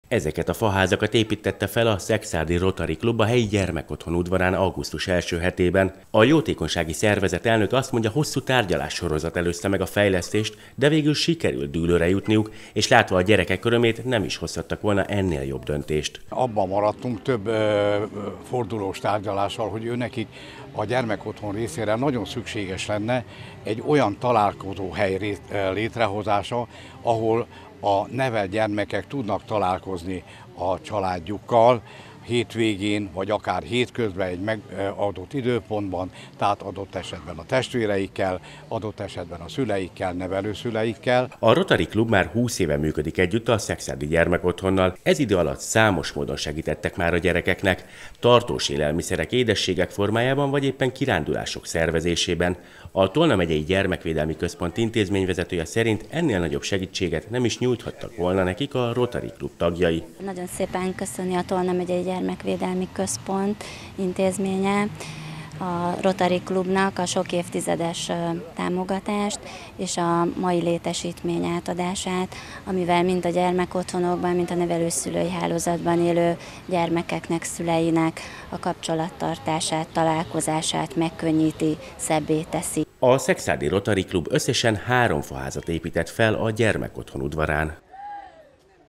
A szekszárdi gyermekotthont fejlesztették a helyi Rotary Club tagjai. 20190809
A szekszárdi gyermekotthont fejlesztették a helyi Rotary Club tagjai. A jótékonysági szervezet önkéntesei faházakat építettek az intézmény udvarára, ahol a gyerekek kulturált körülmények között tudnak a szüleikkel találkozni.